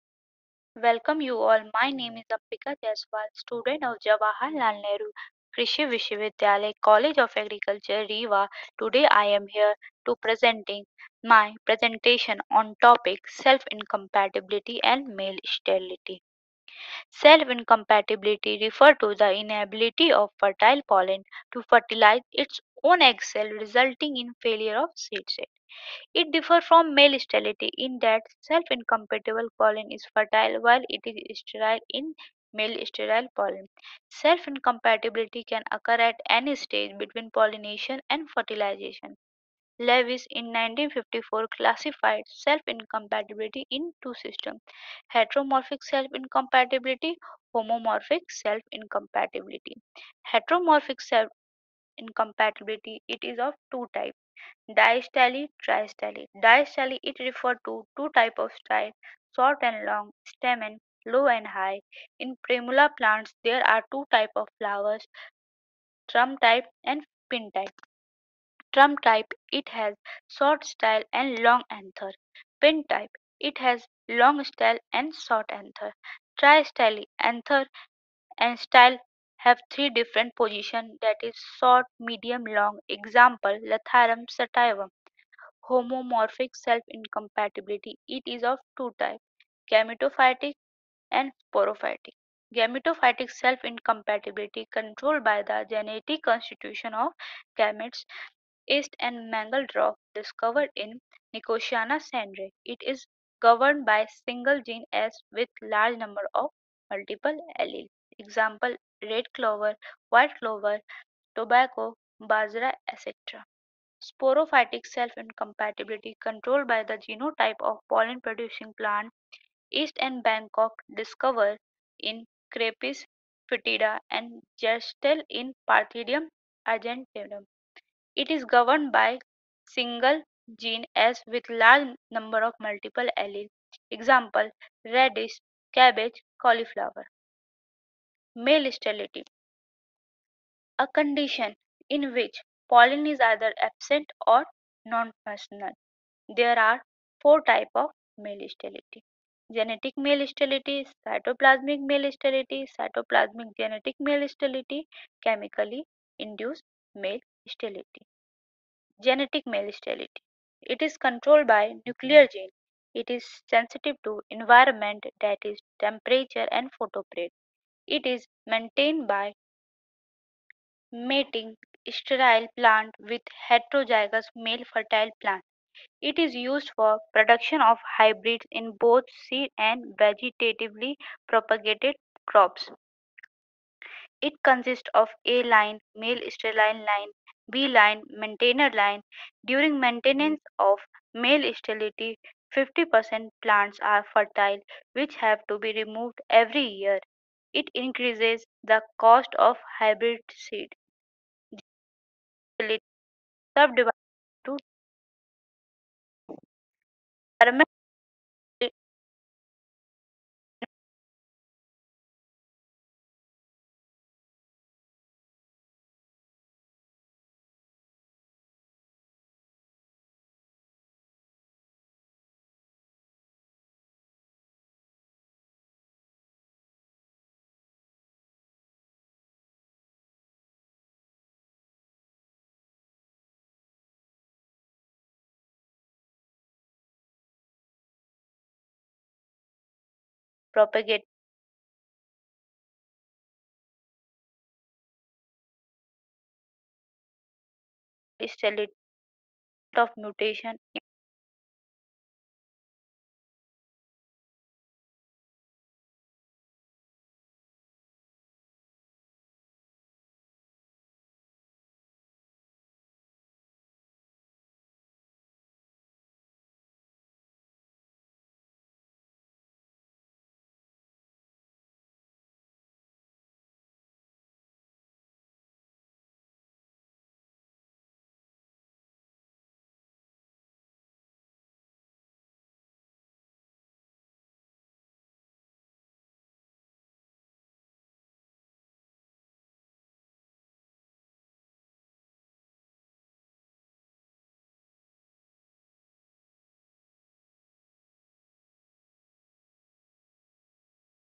Self Incompatibility & male sterlity Ambika Jaiswal BSc Ag 4th Yr JNKVV College of Agriculture Rewa
Document from Dr R P Joshi